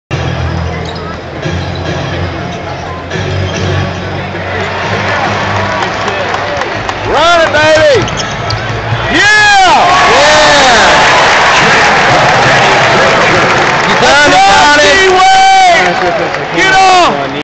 Jarret Jack scores
Jarret Jack scores against the Heat.